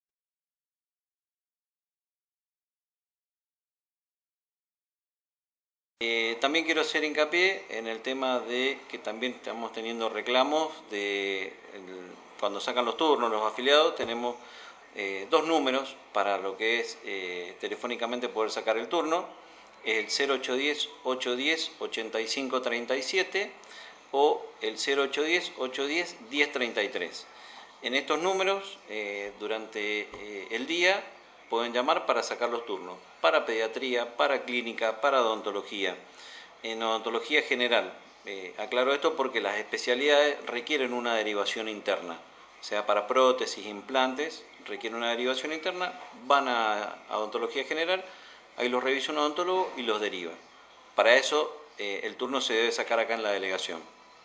SACAR TURNOS MÁS FÁCIL
Hay nuevos teléfonos para sacar turnos en OSEP. Copiarlos y tener a mano.